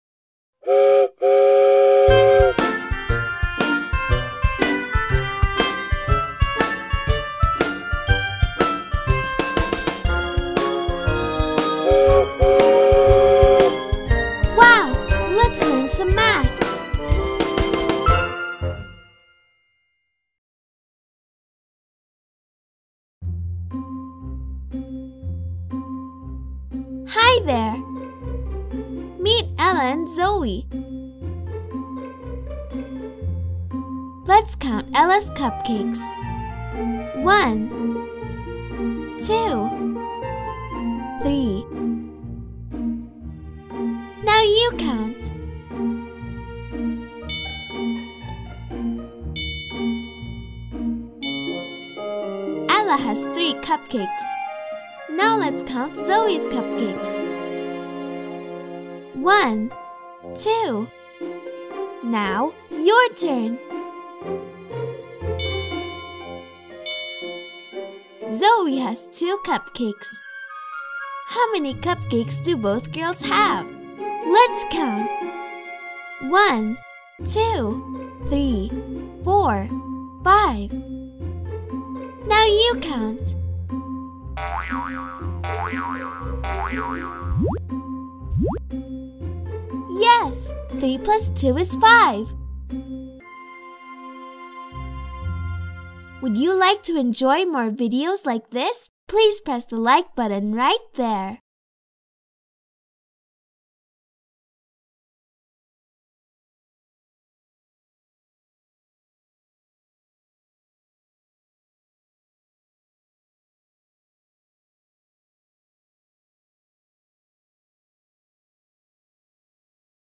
Cupcakes Math - Addition: 3 + 2
This video is great for learning how simple addition works. If you would like to see more videos like this, please "like" this one :)